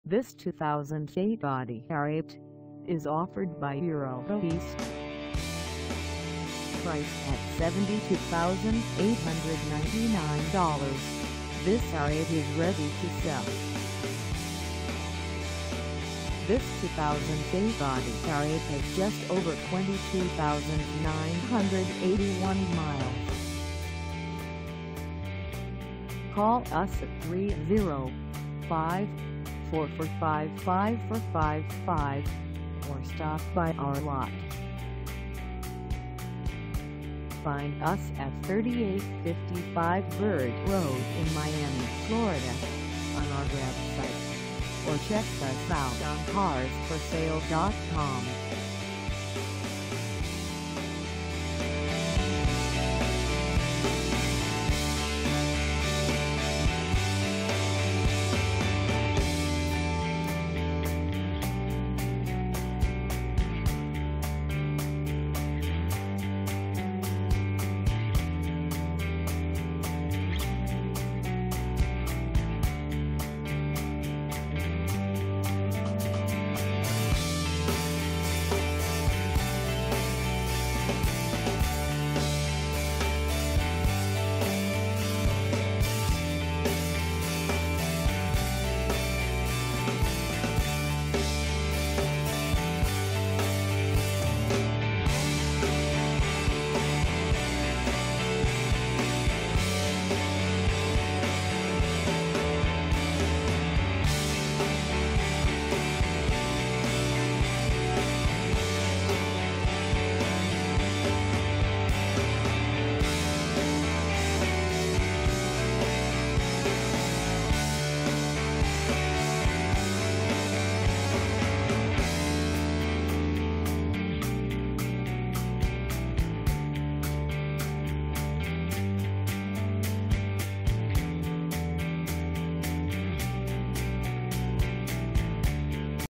2008 Audi R8 quattro for sale in Miami, FL 33146 at Eurotoys
This 2008 Audi R8 quattro is for sale in Miami, FL 33146 at Eurotoys.

Engine: 4.2L V8
Transmission: Automatic 6-Speed
Color: Gray
Mileage: 22981
Contact Eurotoys 3855 Bird Road Miami, FL 33146 to test drive this 2008 Audi R8 quattro today.